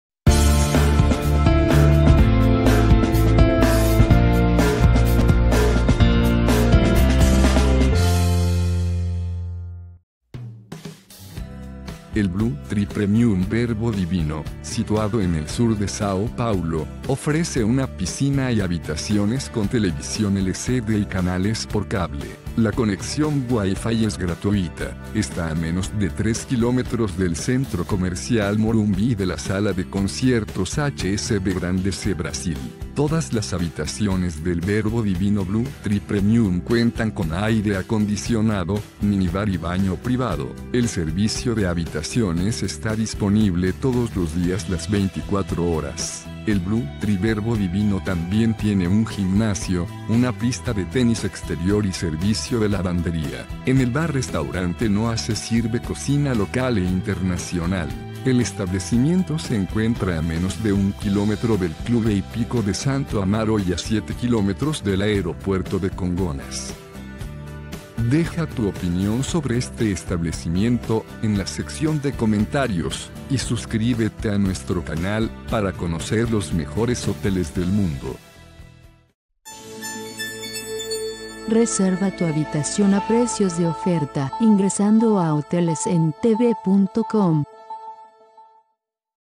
Hotel Blue Tree Premium Verbo Divino en Sao Paulo
Blue Tree Premium Verbo Divino incluye restaurante, servicio de habitaciones y bar. El edificio de Blue Tree Premium Verbo Divino se encuentra ubicado en Rua Verbo Divino, 1323 y el equipamiento de sus 380 habitaciones se compone de minibar, ducha y caja fuerte.